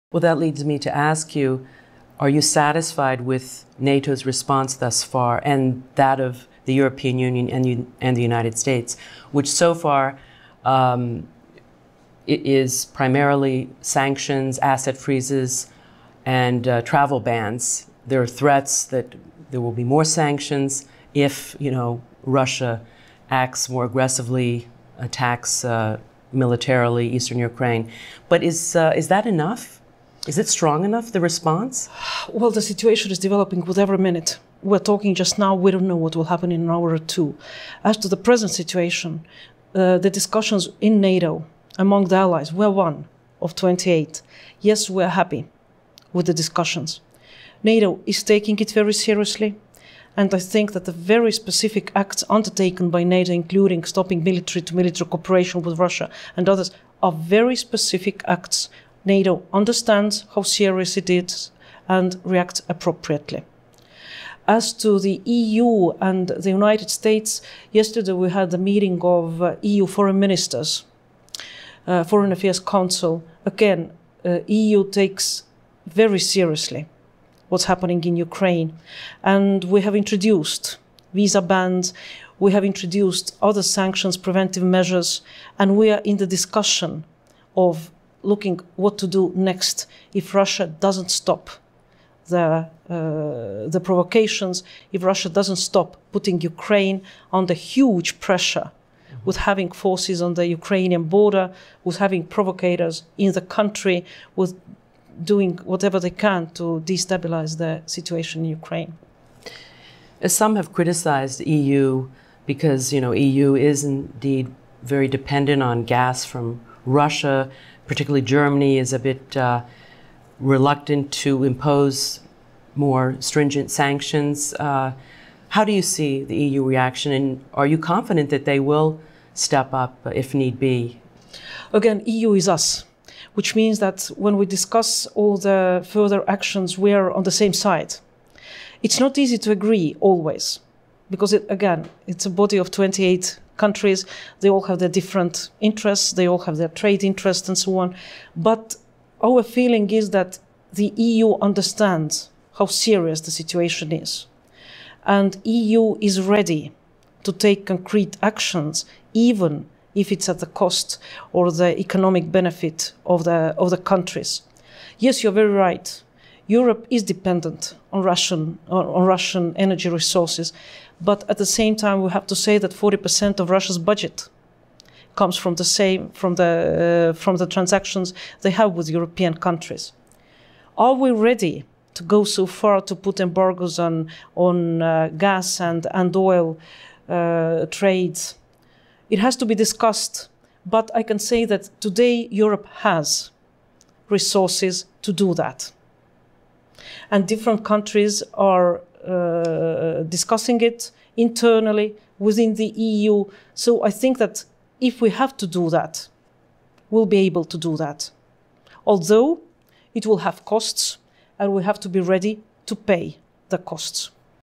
Ambassador Marina Kaljurand on the EU & US Sanctions
On this edition of the program, a wide-ranging conversation with Estonian Ambassador to the U.S., Marina Kaljurand, about Russia's provocative moves in Ukraine -- from the annexation of Crimea to its backing of separatists in the East. Estonia is a member of both the European Union and NATO. Ambassador Kaljurand tells host Carol Castiel that Moscow's actions constitute a blatant violation of international and legal norms and she worries about implications for Estonia and other Baltic states, which have sizeable Russian minorities. Ambassador Kaljurand discusses what might be motivating Moscow's behavior and comments on the response to Russia's actions by the United States, the EU and NATO.
For update on Press Conference USA, and other VOA's programs, follow host Carol Castiel on Twitter: @CarolCastielVOA